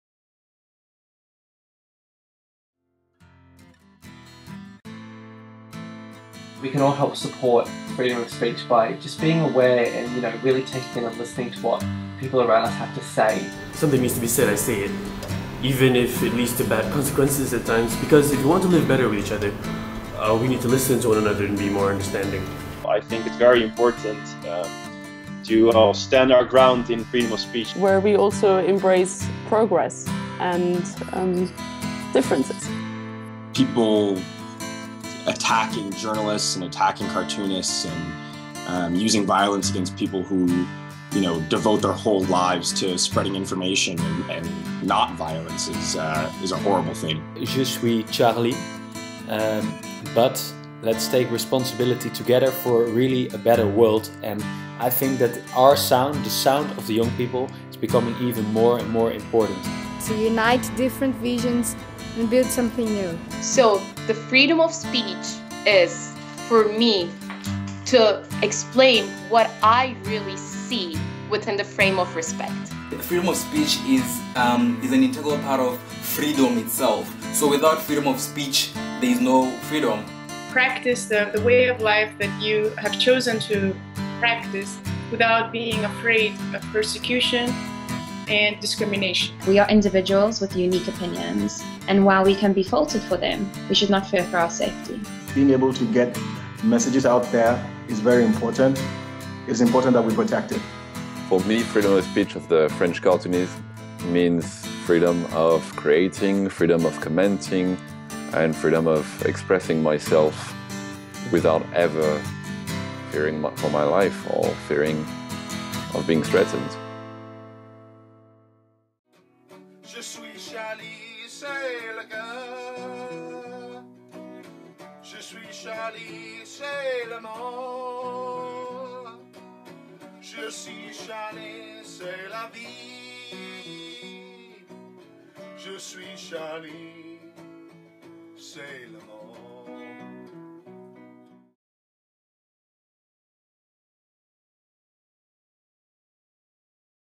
We Call for Freedom of Speech
In this special vlog multiple intoConnection vloggers take a stand for freedom of speech. Due to the current affairs in Paris, we had to re-think what freedom of speech means. Because it doesn’t seem so normal anymore after the shootings at satirical weekly Charlie Hebdo. So we searched for quotes that have been made in the past by inspiring people and it feels as if they were made today. We asked our vlogger community what freedom of speech means to them. Young people from all over the world shared their thoughts. Turns out that what has been said in the past doesn’t differ much from what is said today. Keep fighting for your spoken and written word, it’s stronger than any form of violence.

Together we build a brighter future.